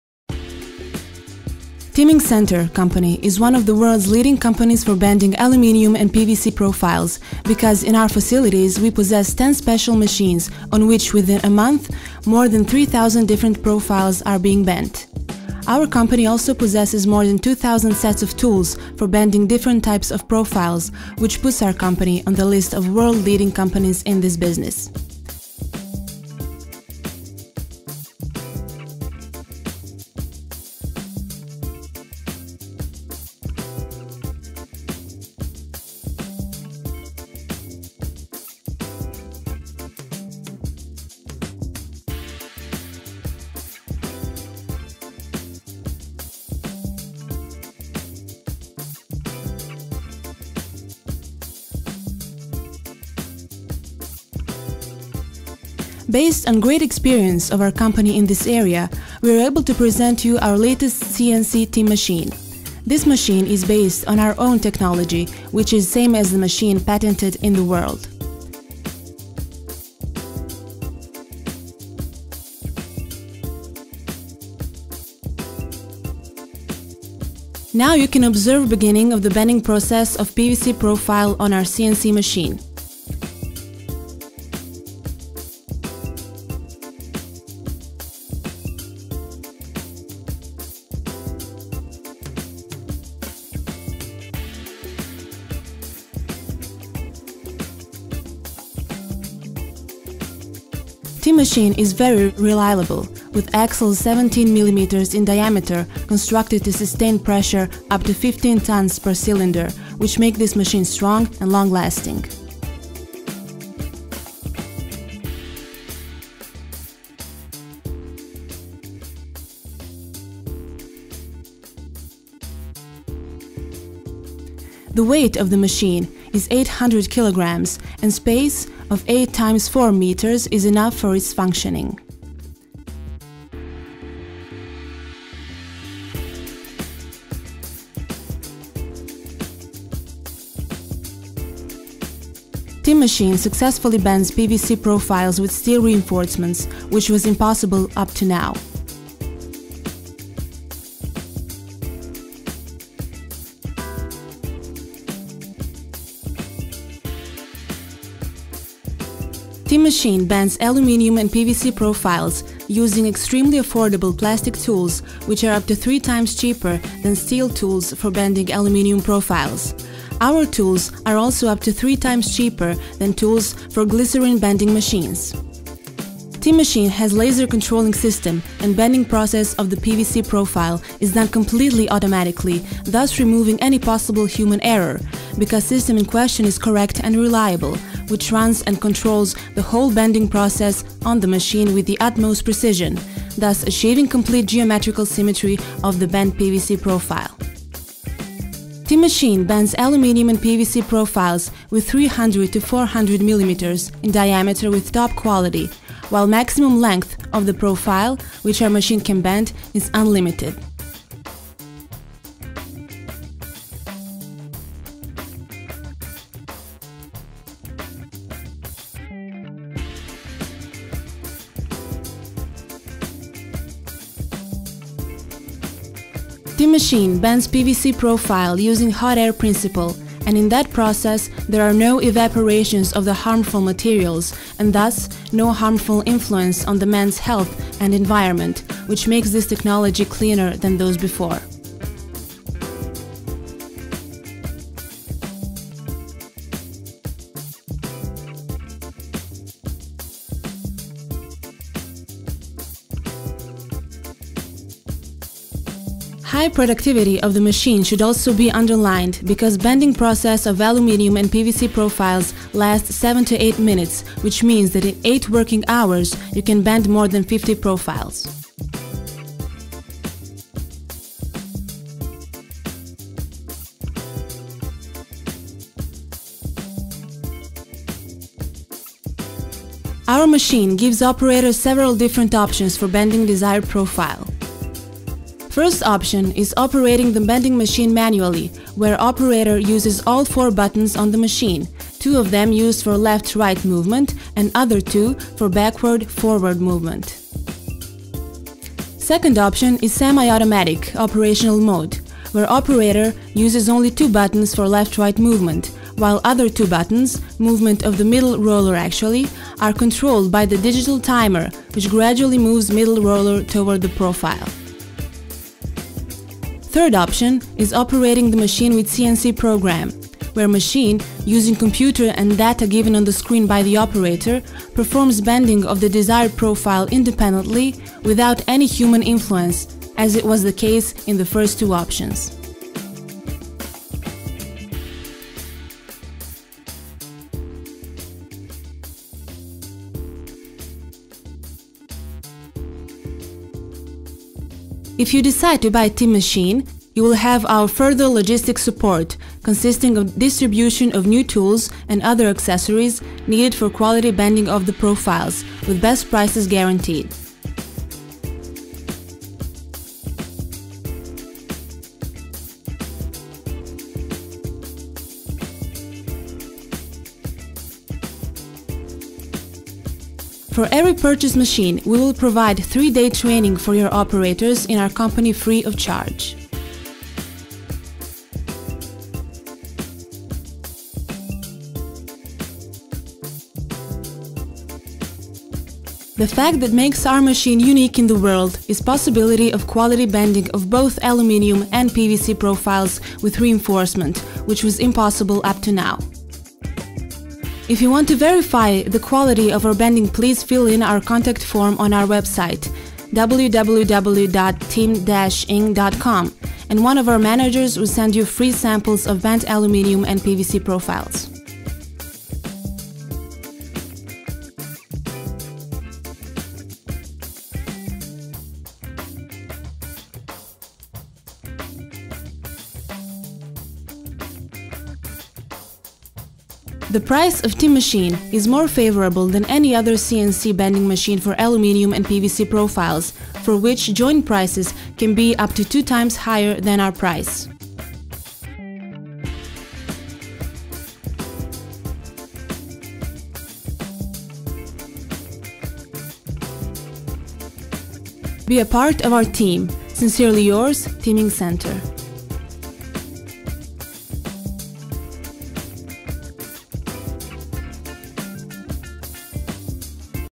Bending machine for PVC profiles (real time bending)
Realtime PVC profile bending on TIM-MACHINE.

TIM-MACHINE Bending machines for Aluminium and PVC profiles in action. Machines built by Tim-Ing Centar Kragujevac, Serbia.